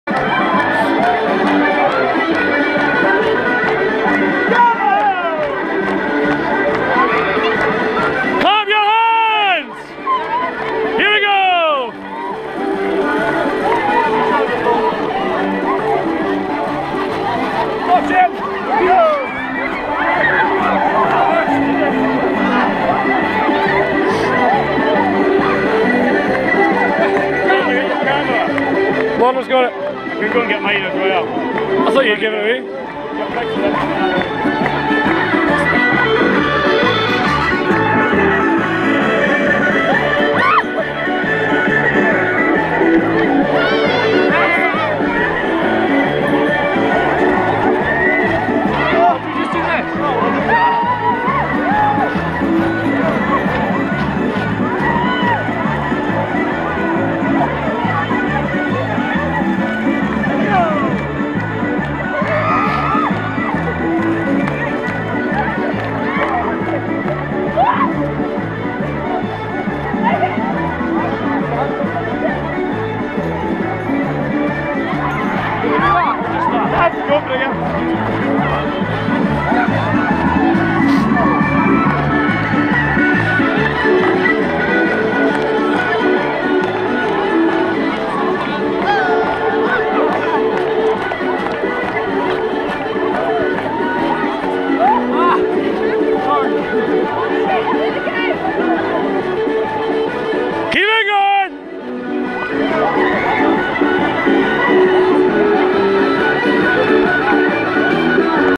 Grantown Grammar Santa Hooligans Jig...
Santa Fun run warm up, decided to do a whole school hooligans jig...